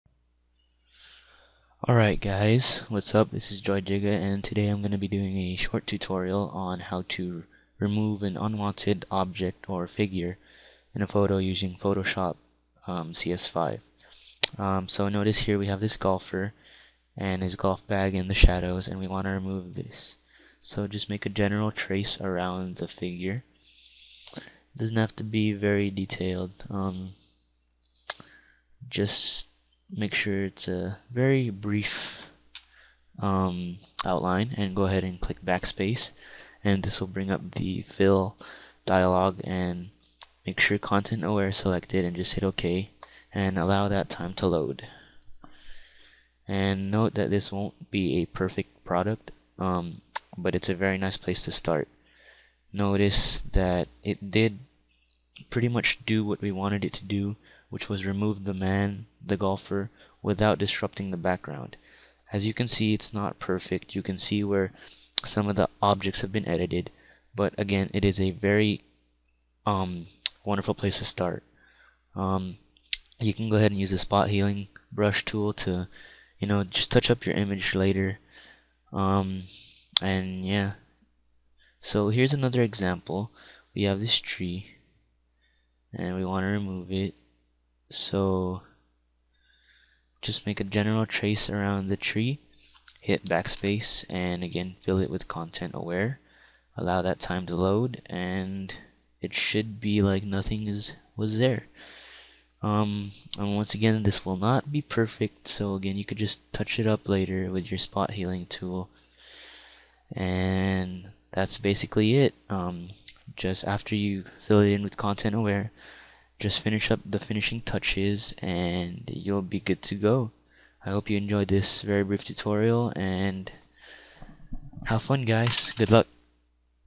Adobe Photoshop CS5: Content-Aware Fill Tutorial [HD]
A brief lesson on how to use the Content-Aware fill option in Adobe Photoshop CS5, to quickly remove unwanted objects in our everyday pictures.

For more info on how to get Adobe products, please